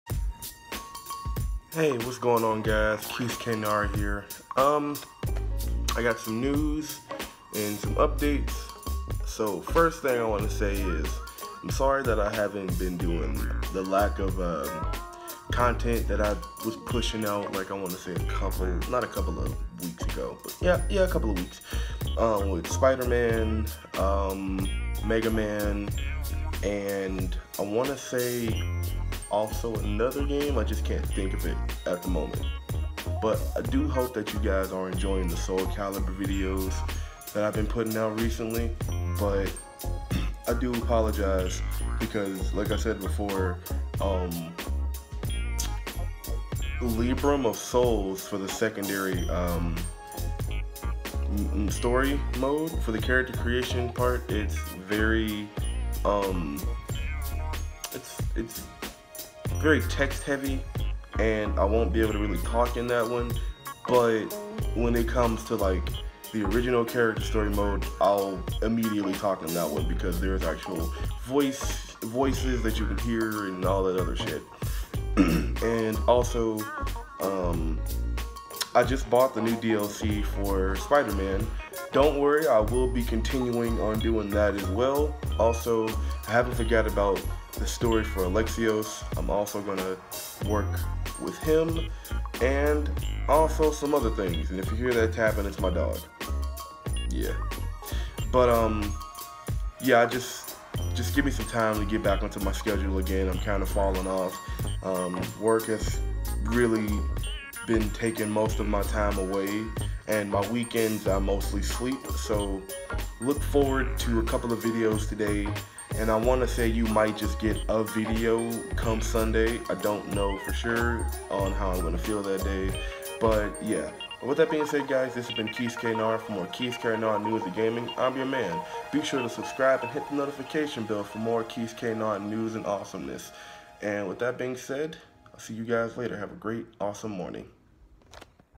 Kisuke Nara update
So yeah I’ve slacking off once again on uploading fear not tho I’m not asking for like pity or anything just letting you guys know I done fucked up yet again shit happens and personally my job is mostly the hold up for the vids being so weak an non consistent sorry about that folks